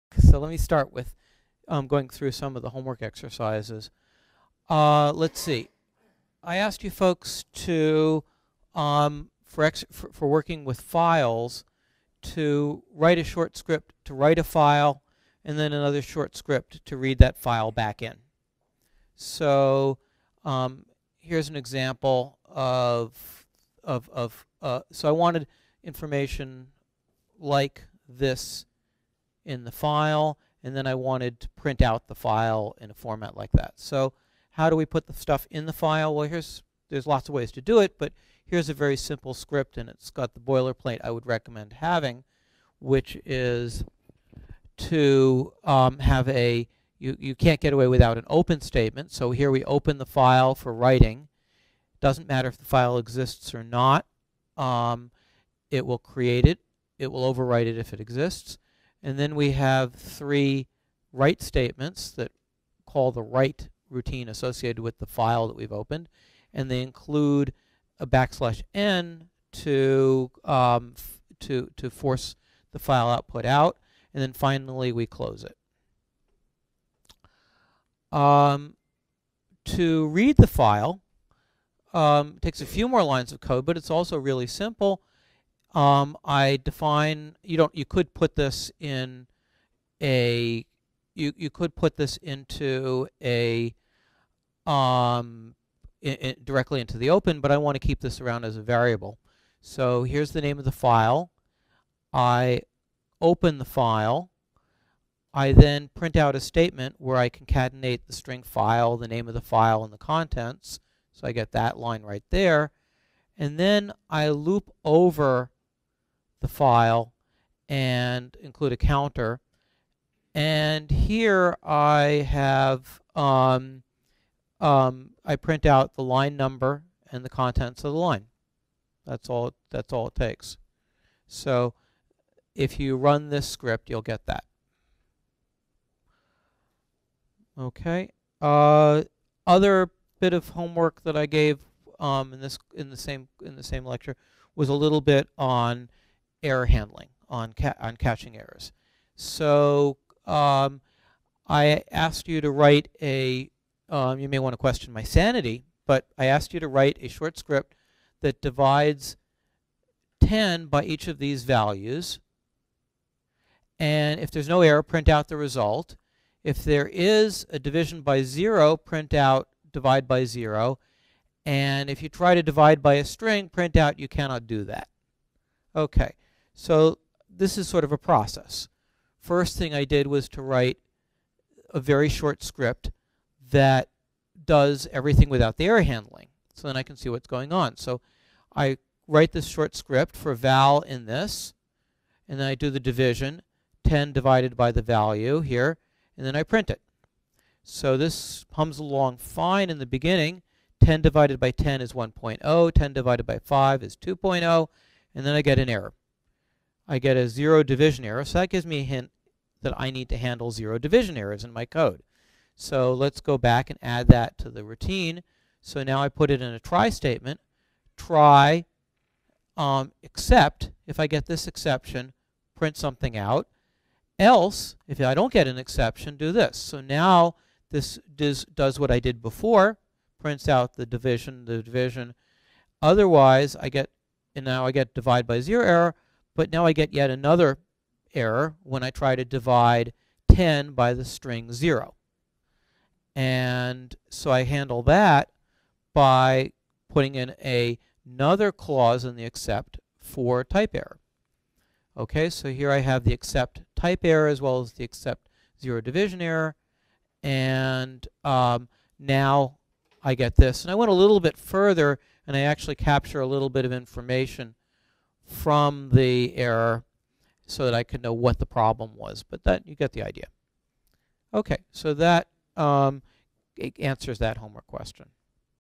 Learning Python - Homework 3
The AES Software Services Group at Argonne National Laboratory hosted a series of Python programming tutorials. Focus is on using Python for synchrotron beamline and scientific programming.